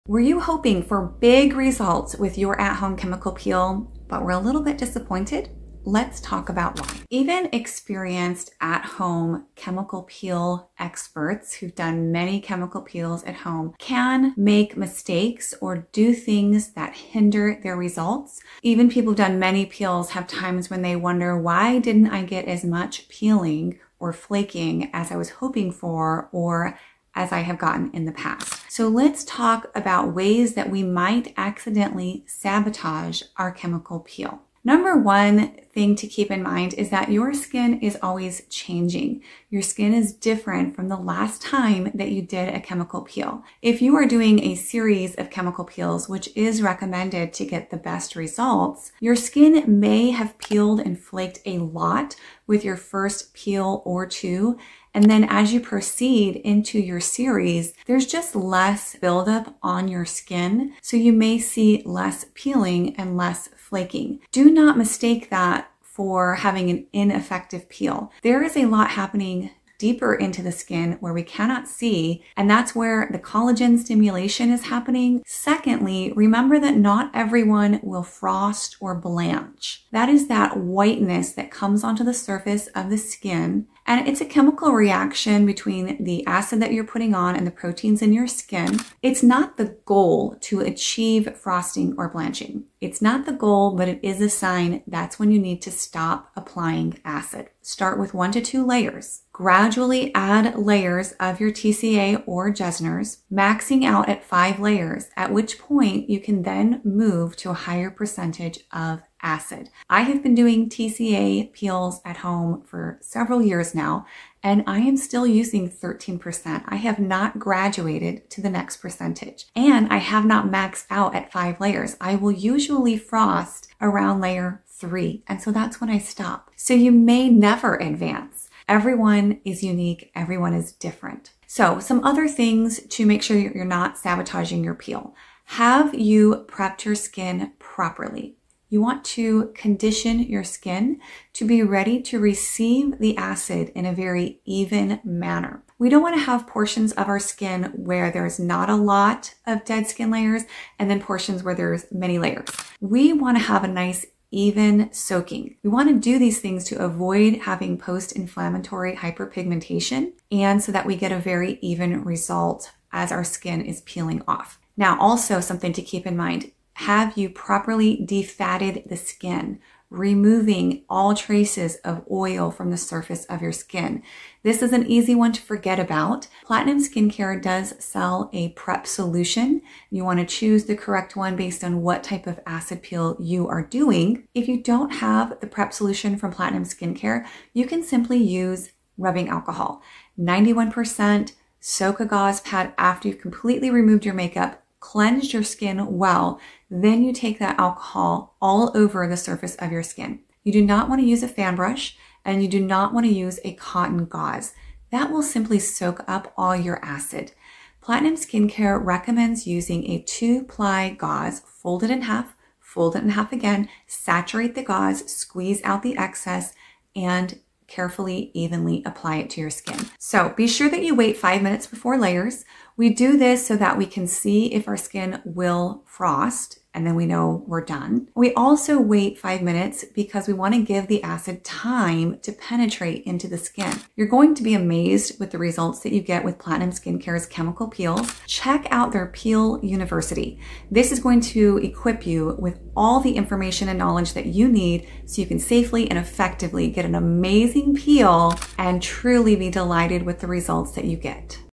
Why Your Chemical Peel Isn't Working (And How to Fix It)
📺 About This Episode:
Did your at-home chemical peel not give you the results you expected? Even experienced peel users can make small mistakes that sabotage their results.

In this video, Kayte breaks down the most common reasons why chemical peels underperform — and exactly how to fix them. From proper skin prep to understanding why your skin might not frost, you'll learn how to get the best possible results from every peel.

✨ Whether you're a first-timer or a seasoned pro, these tips will help you maximize your peel results safely and effectively.

🧴 What You'll Learn:
✅ Why your skin changes between peels (and why less flaking doesn't mean less results)
✅ The truth about frosting and blanching — it's not the goal, but it is your stop sign
✅ How to properly prep your skin for even acid penetration
✅ Why defatting your skin is crucial (and how to do it right)
✅ The correct tools to use (hint: ditch the fan brush!)
✅ Why waiting 5 minutes between layers matters
✅ How to gradually build up layers and percentages safely
✅ What's happening deeper in your skin even when you can't see dramatic peeling

💬 Let's Talk:
Have you ever been disappointed with your peel results? What questions do you have about getting better peeling and flaking? Drop your questions in the comments — we're here to help!